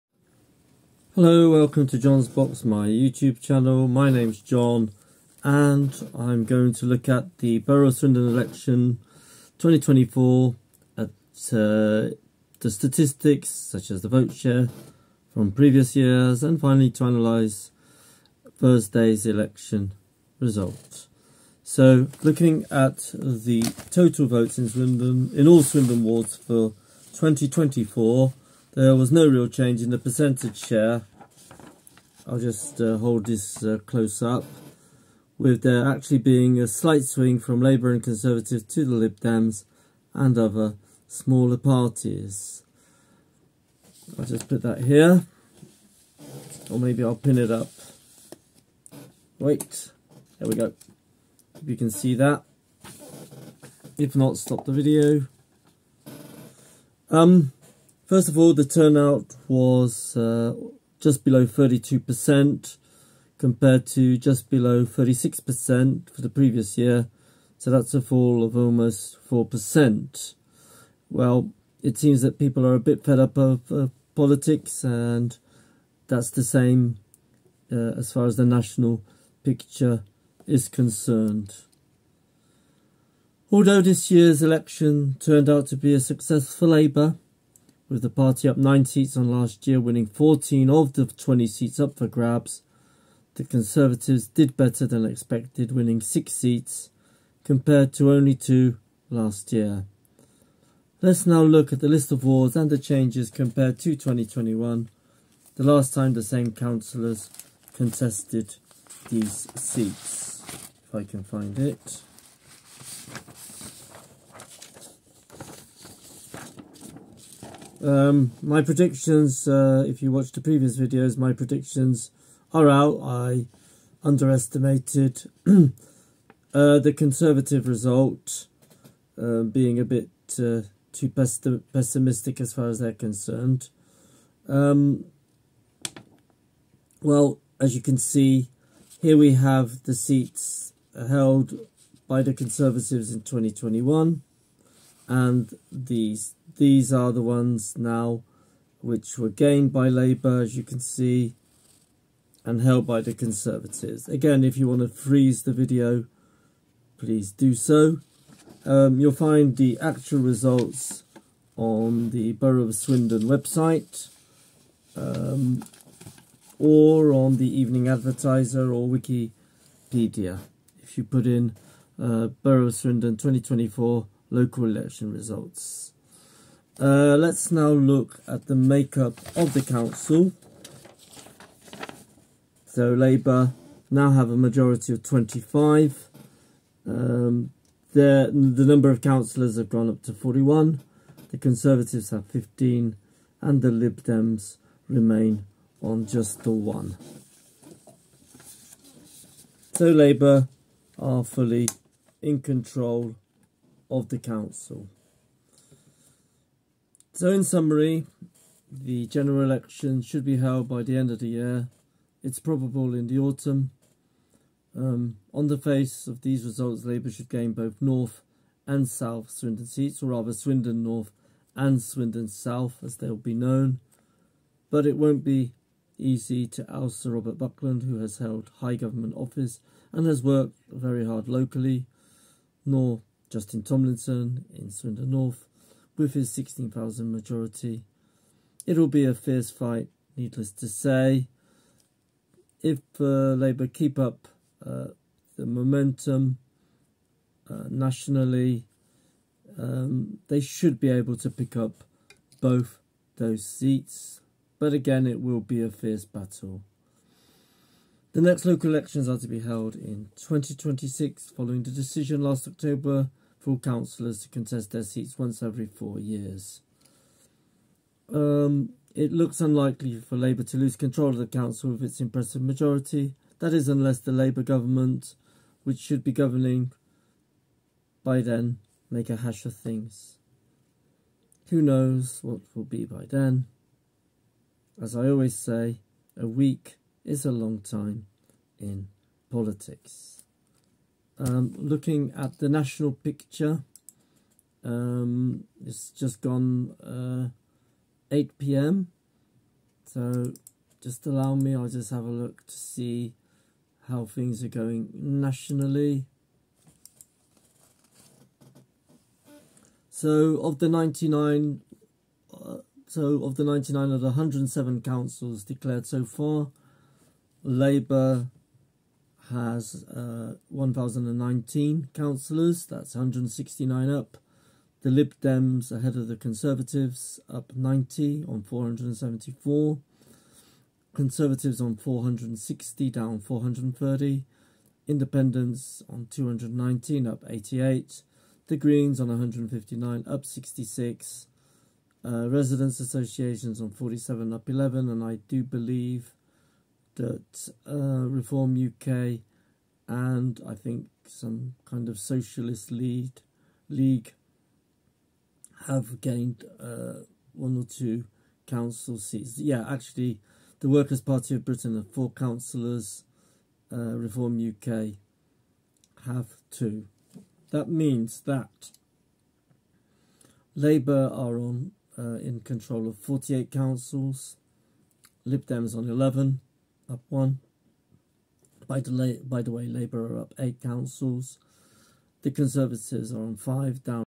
SWINDON Local ELECTIONS 2024 - What the results mean
The local elections took place on 2nd May 2024. Labour increases its majority on the council, but what do the results really mean and what are Labour's chances of gaining the two Swindon seats from the Conservatives?